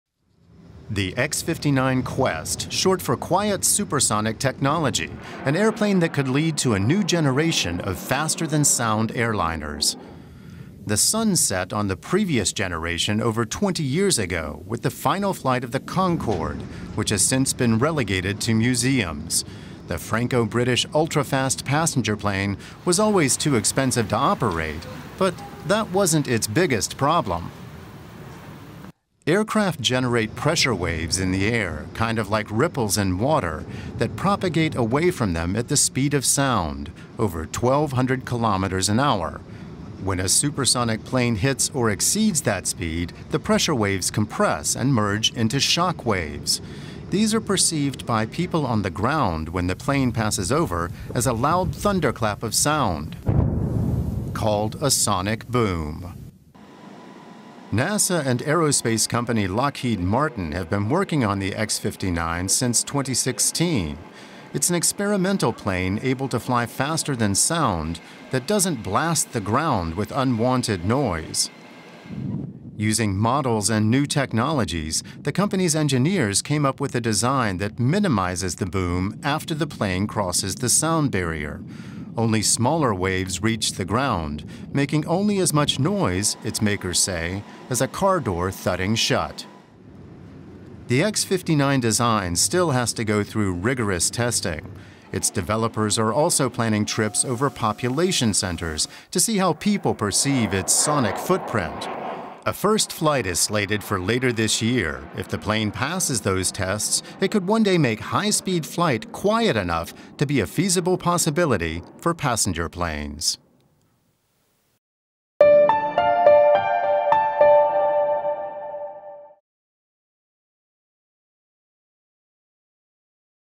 'Can Fly Faster Than Sound:' NASA Unveils Future of Supersonic Flight | Watch
Lots of fighter jets have top speeds more than twice the speed of sound, but flying that fast is banned for airliners because it creates a sonic boom. Now NASA is rolling out a quiet experimental aircraft that could help put ultrafast commercial air travel back on the table. Watch this to know more.